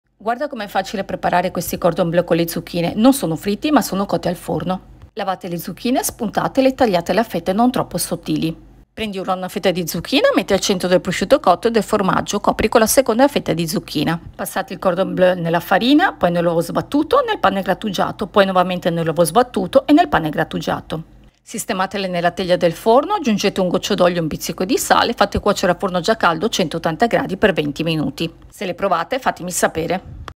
Cordon bleu di zucchine
Cordon bleu di zucchine tonde, sono buonissimi e sono cotti al forno, li preparate con facilità e sono un secondo piatto perfetto anche per i bambini. Per preparare le zucchine in carrozza, potete usare i formaggio a pasta filata che più preferite, scamorza, provola o anche della mozzarella per pizza. Oltre ai Cordon bleu di zucchine farcite con prosciutto e formaggio vi consiglio di provare anche i cordon bleu di melanzane anch’essi gustosi e facili, magari li potete preparare entrambi nella stessa teglia. Ora andiamo a vedere come fare i cordon bleu con le zucchine tonde e se li provate fatemi sapere. Barbara

ATTIVA LE NOTIFICHE CLICCANDO SULLA CAMPANELLA PER ESSERE AVVISATO OGNI VOLTA CHE PUBBLICO UNA NUOVA RICETTA

INGREDIENTI :
2 zucchine tonde
100 g prosciutto cotto
100 g formaggio a pasta filata (provola, scamorza, formaggio per pizza)
2 uova
q.b. farina
q.b. pane grattugiato
q.b. sale